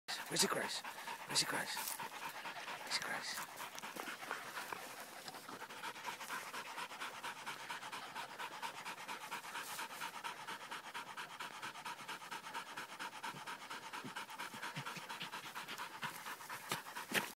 West Highland Terrierr going to ground
Gracie checking it out after a dig :-)